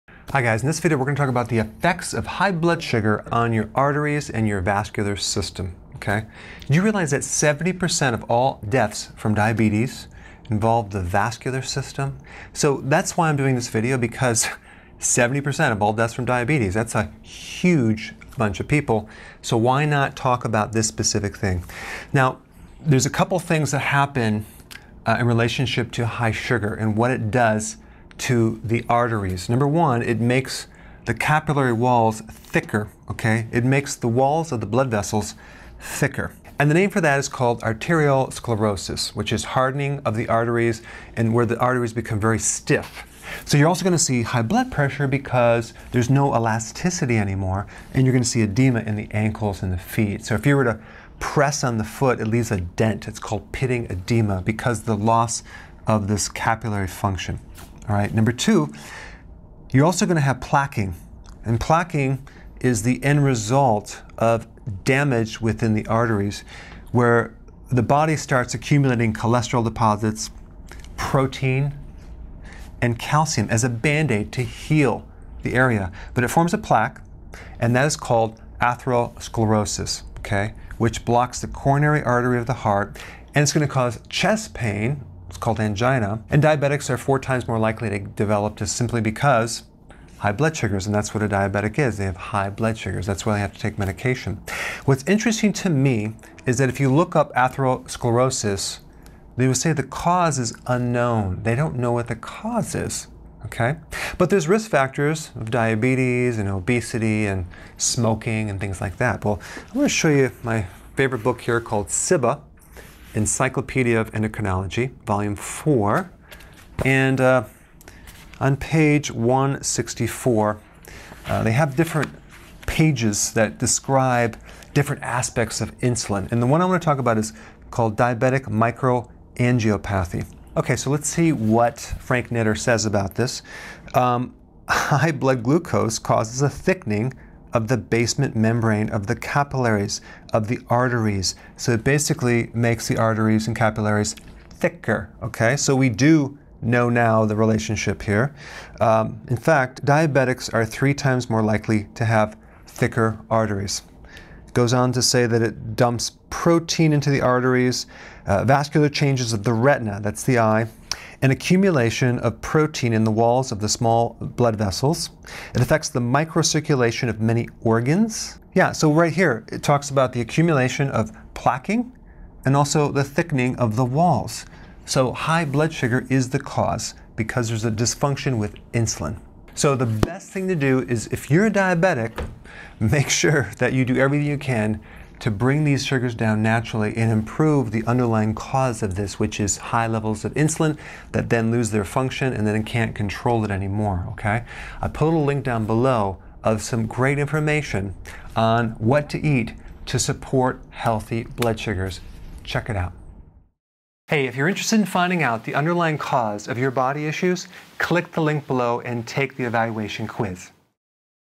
The Effects of High Blood Sugar on Arteries – Dr. Berg on Arteriosclerosis & Atherosclerosis
Dr. Berg talks about the effects of high blood sugars on the arteries. 70% of all deaths from diabetes

1. High sugar levels in the blood make the arteries thicker - this is called arteriosclerosis. This is stiffening of the arteries and thus the high blood pressure and edema in the lower legs and ankles (pitting edema)
2. Then there is plaquing, which is a combo of cholesterol, protein and calcium. The body is forming a bio-band-aid and this is called atherosclerosis. This can relate to chest pain (angina) at a rate of 4x more in diabetes and in, blocking of the coronary artery.